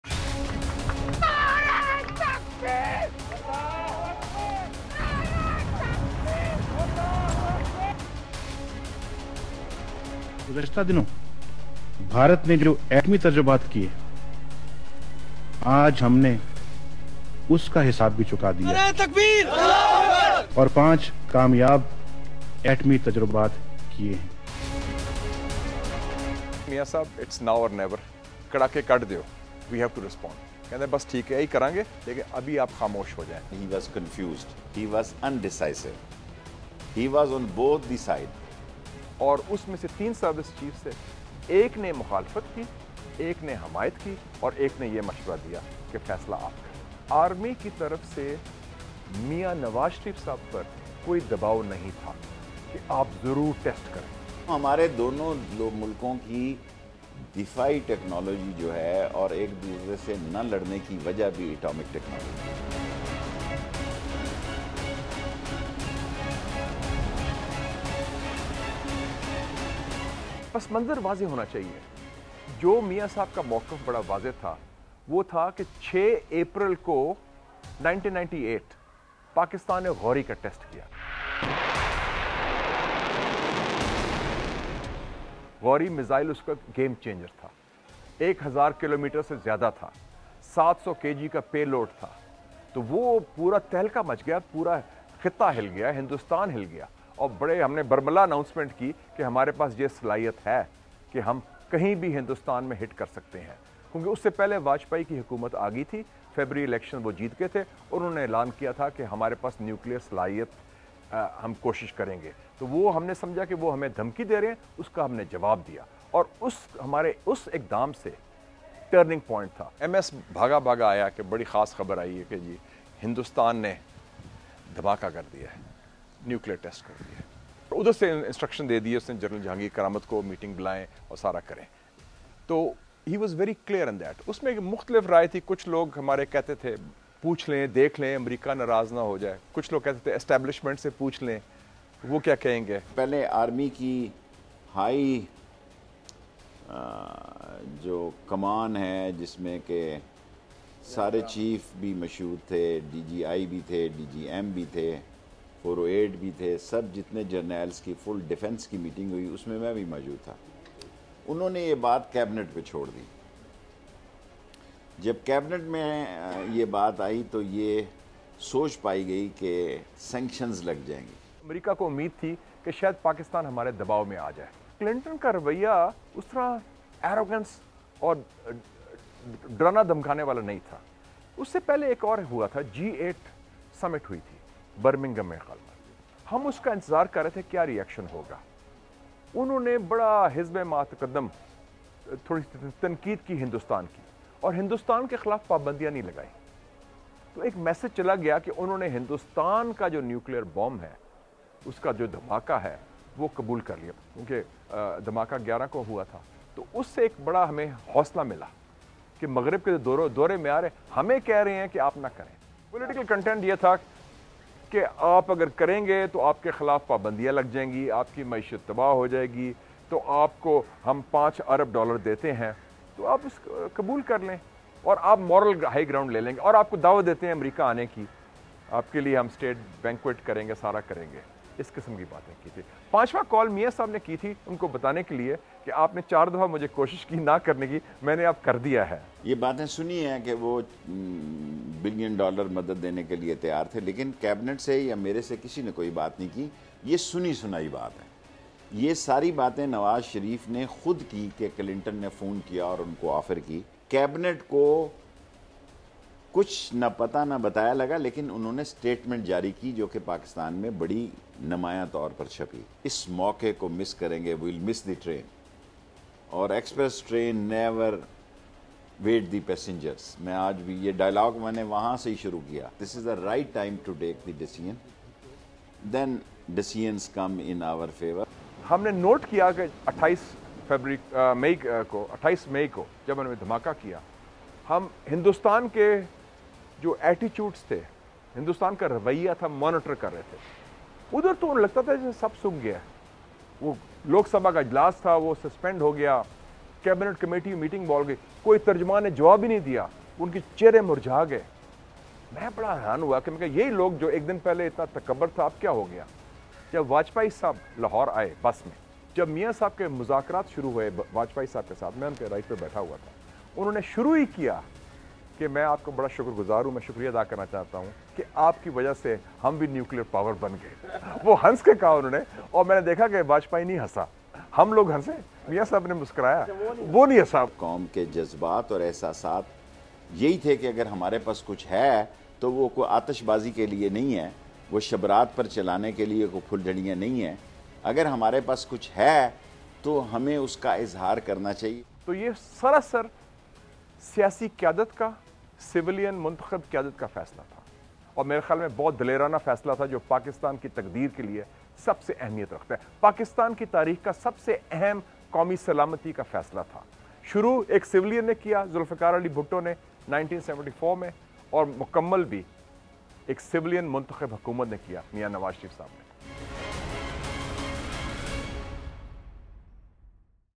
Pakistan's Nuclear Tests: Did the civilian government decide? - BBC URDU
1998 میں انڈیا کے جوہری تجربے کے بعد  نواز شریف کی حکومت کو ایک مشکل فیصلہ کرنا پڑا کہ کیا پاکستان کو بھی ایٹمی دھماکہ کرنا چاہیے؟ 23 سال بعد ہماری رپورٹ میں دیکھیں کہ یہ فیصلہ کیسے لیا گیا؟
ویڈیو: عمر فاروق، نیئر عباس